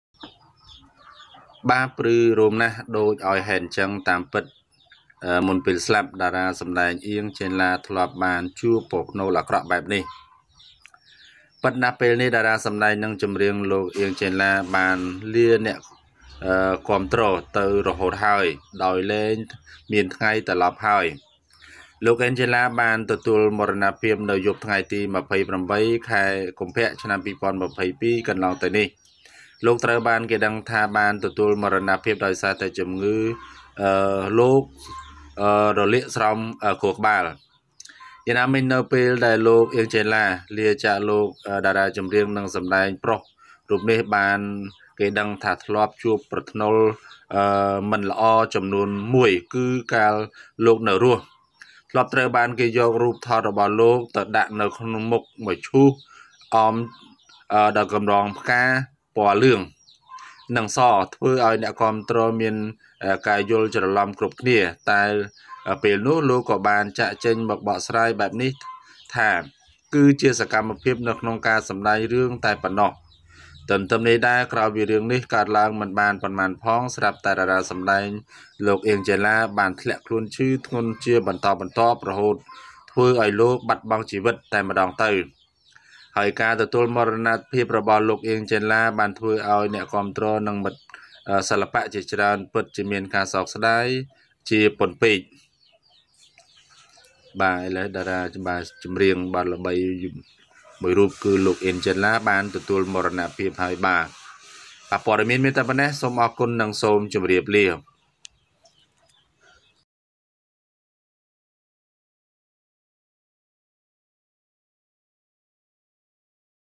Very hairy as the reason why, in fact, before his death, actor Ieng Chenla had such
Very hairy as the reason why, in fact, before his death, actor Ieng Chenla had such a bad…

ព្រឺរោមណាស់ ដូចឲ្យហេតុចឹង តាមពិតមុនពេលស្លា.ប់ តារាសម្តែង អៀង ចេនឡា ធ្លាប់បានជួបប្រផ្នូលអាក្រក់បែបនេះ…